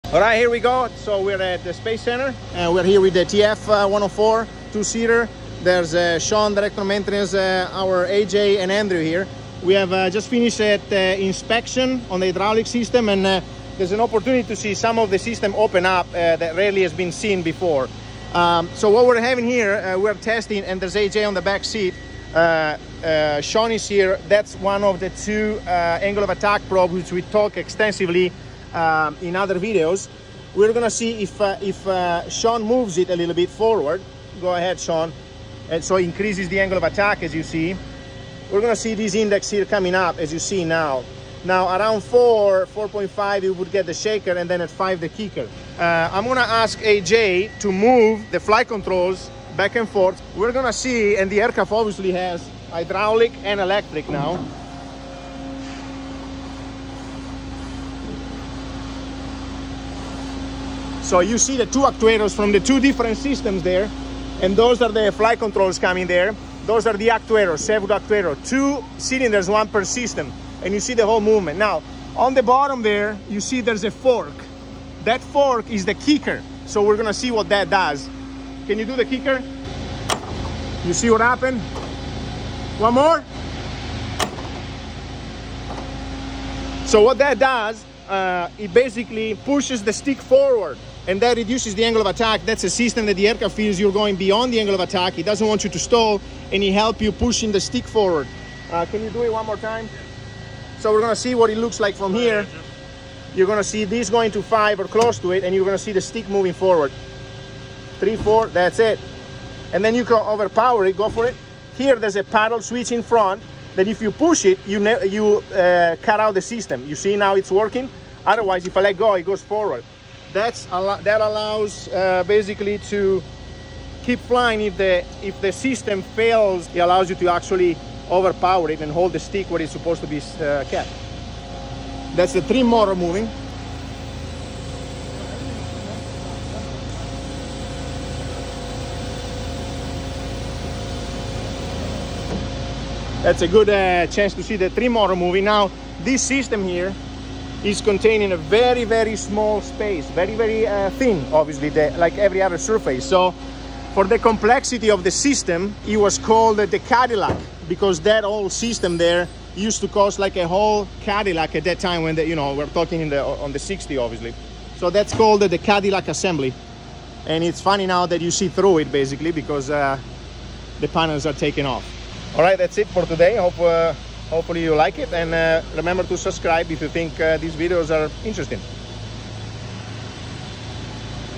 Lockheed F104S ASAM Cadillac Assembly - RARE footage of functioning flight control systems
Aircraft alive on the ground in the hangar, with Hyd and Electric Power supplied.
Rare footage of some of the flight control systems while functioning. Auto Pitch Control system revealed.